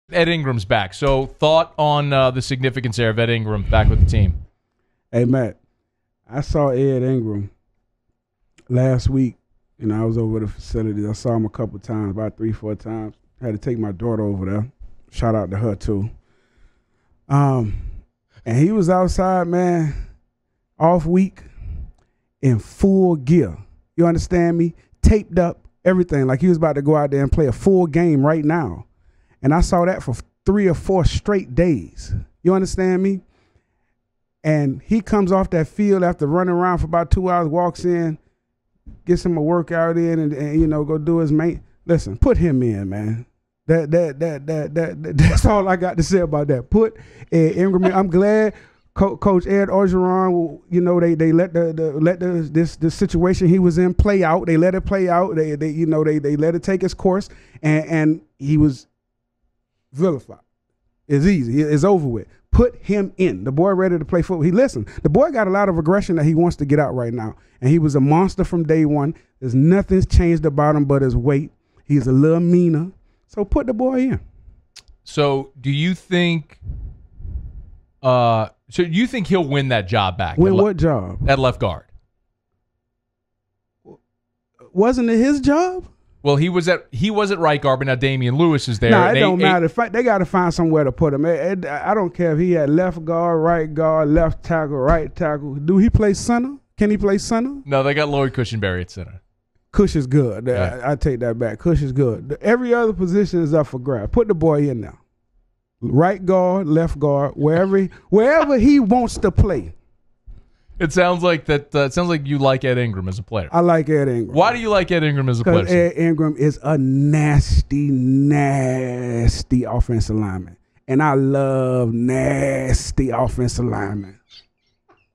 Rohan Davey excited about ‘nasty’ Ed Ingram’s return
Needless to say, Rohan Davey is excited about Ed Ingram rejoining the LSU team 😂😂😂😂